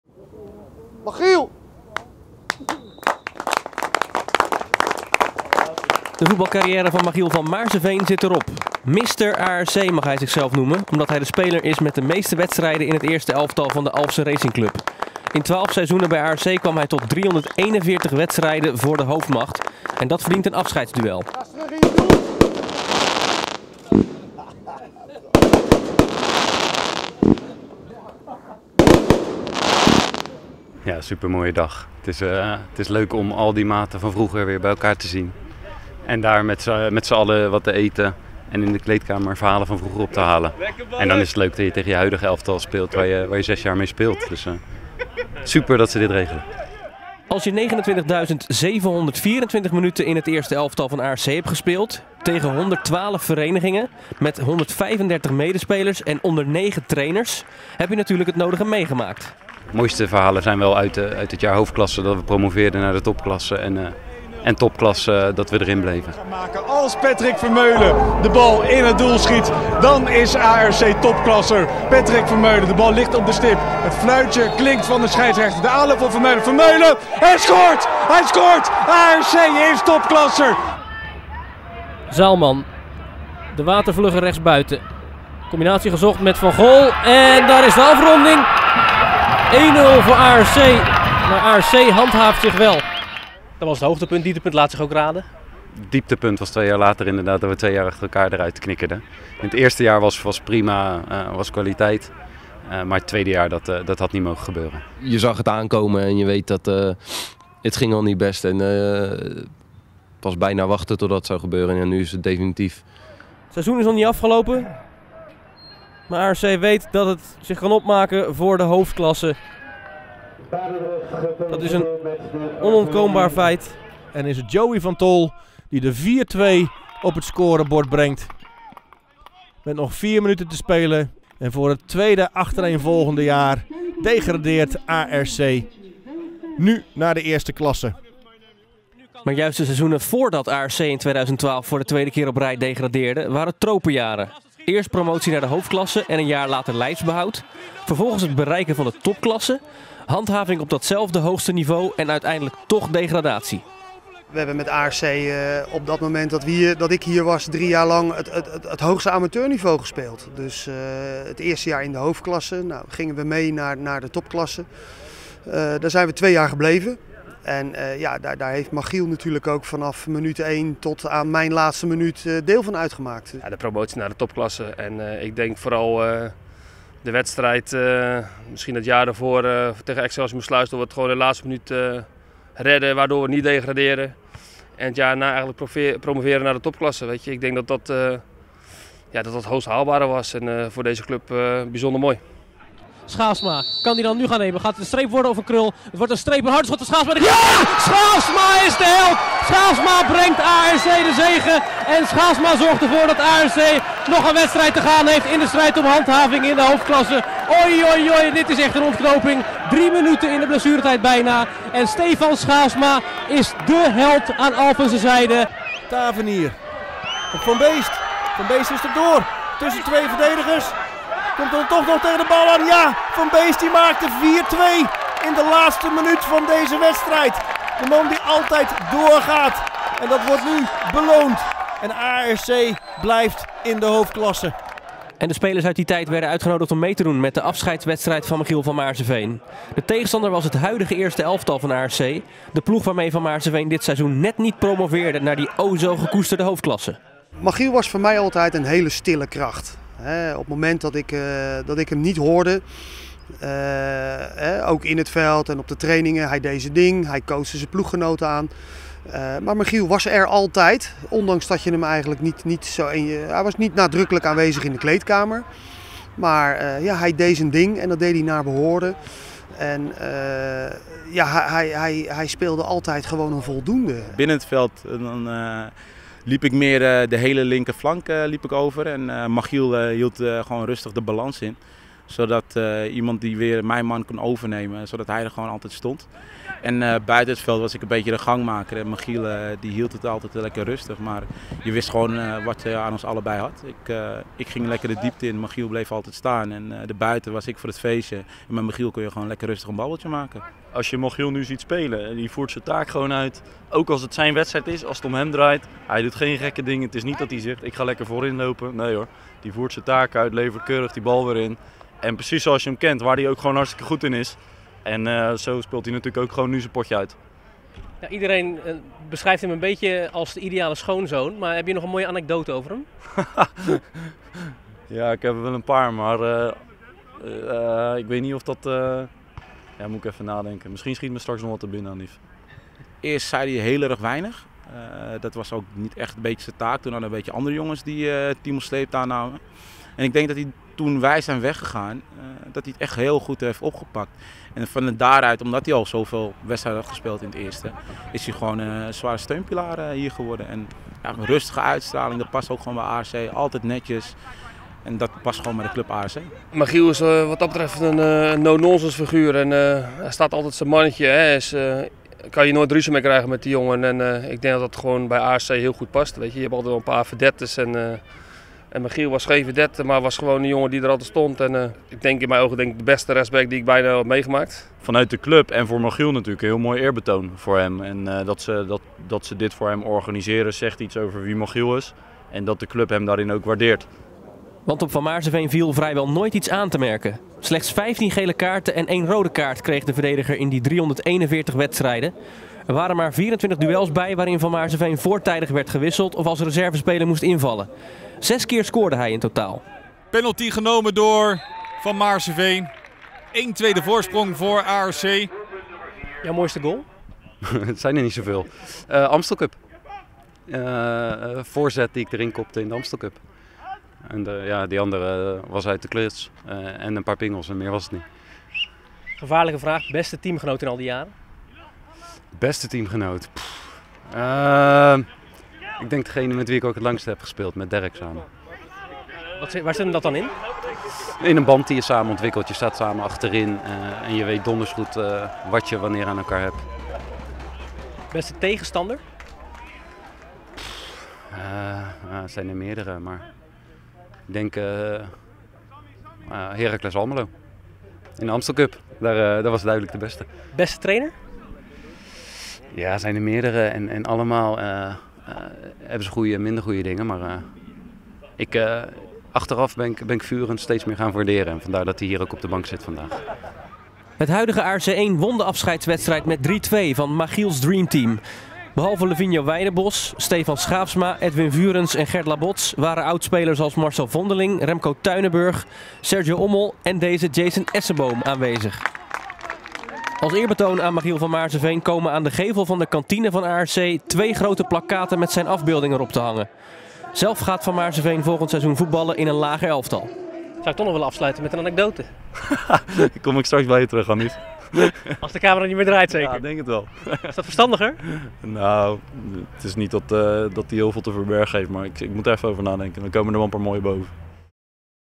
Afscheidswedstrijd 'Mister ARC' Machiel van Maarseveen
In twaalf seizoenen tijd speelde Machiel van Maarseveen 29.724 minuten in het eerste elftal van ARC. 341 wedstrijden tegen 112 verschillende verenigingen, met 135 medespelers en onder leiding van negen trainers. Vier seizoenen kwam de rechtsback uit op het allerhoogste amateurvoetbalniveau van Nederland. Er is geen speler in de clubgeschiedenis van ARC geweest die zo vaak in ARC's hoofdmacht speelde als Van Maarseveen. Drie weken geleden speelde de verdediger (32) zijn laatste officiële wedstrijd, de Alphenaar stopt met voetballen.